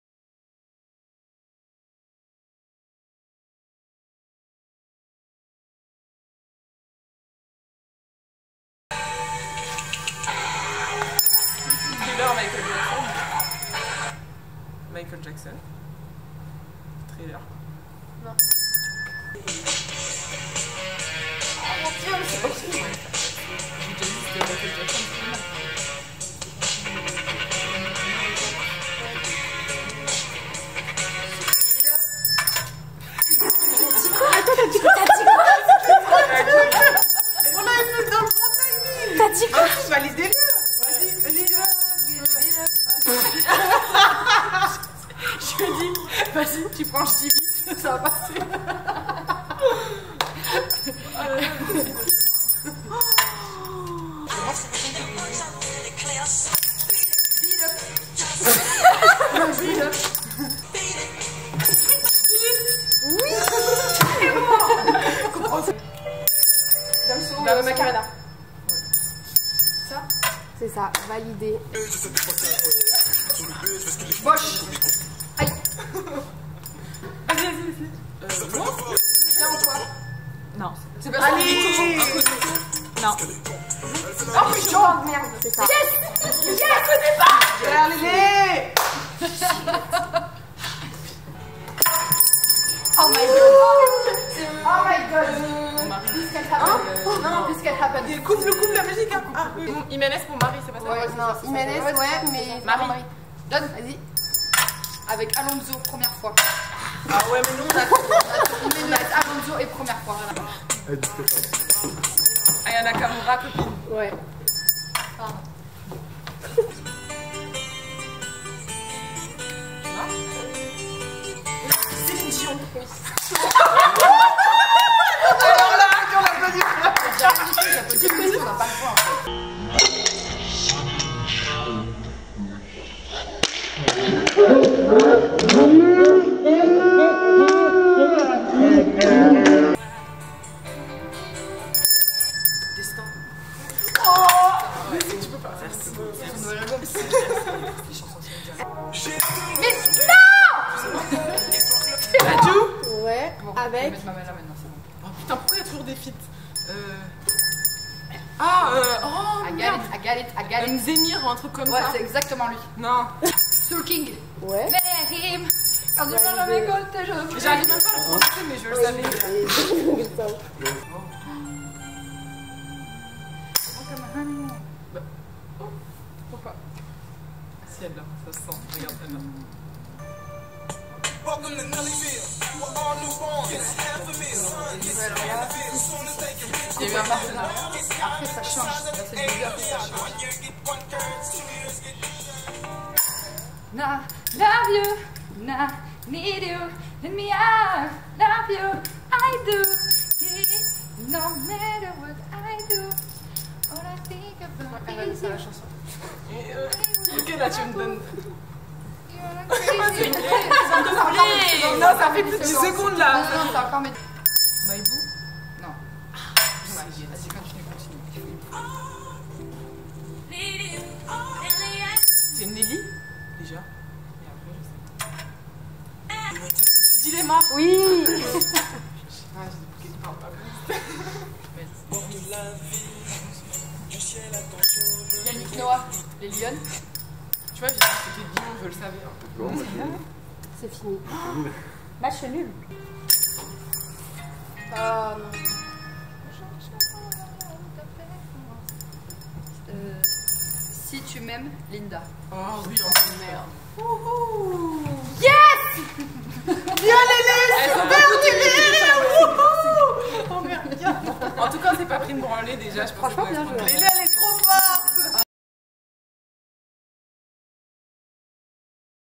Blind test #2
Retrouvez la seconde partie du Blind Test !
Quelle équipe ressort gagnante de ce match ?